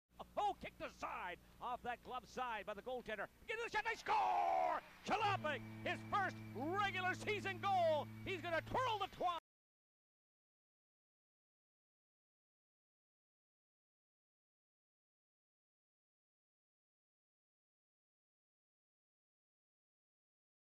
Goal of the Game: Filip Chlapik's first Islanders goal
Relive Filip Chlapik's first regular season goal as a member of the Charlottetown Islanders.